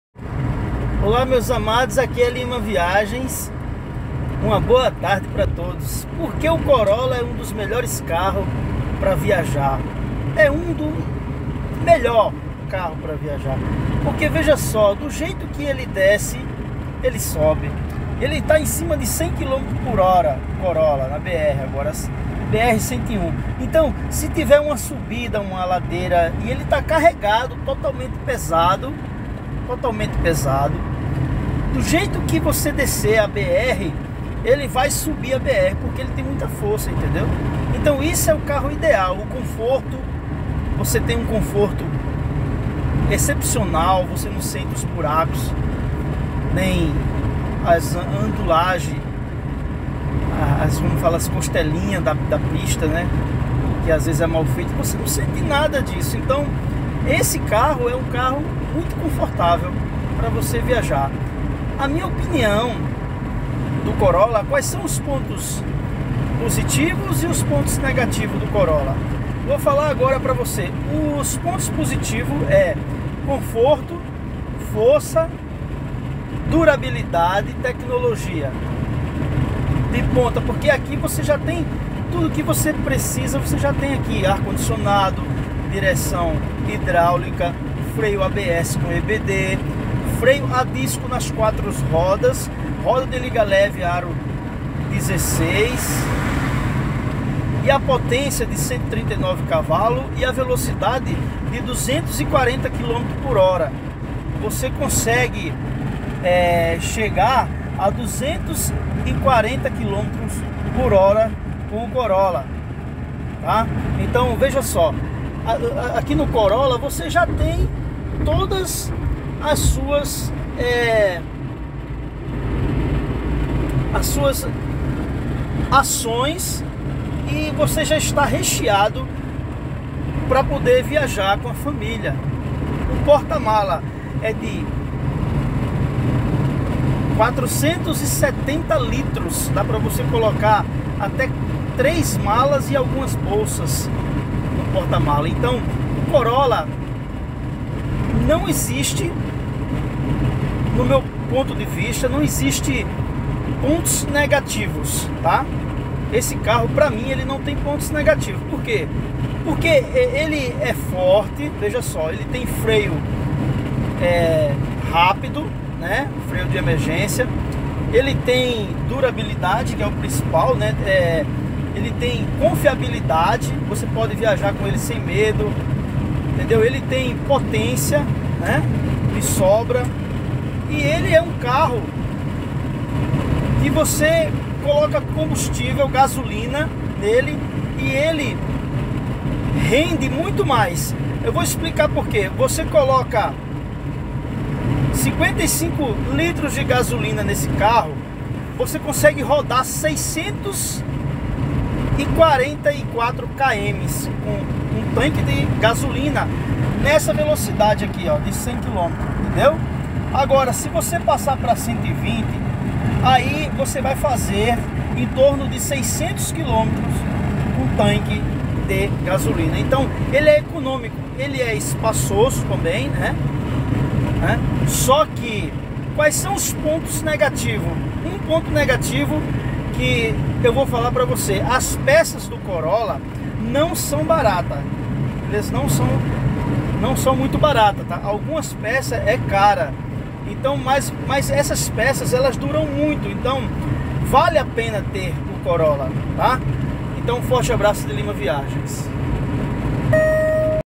PONTOS POSITIVOS , E PONTOS NEGATIVOS COROLLA BRED PIT .....🇧🇷
Pontos positivos do corolla na br como ele se comporta na estradas ruínas do brasileira. 🇧🇷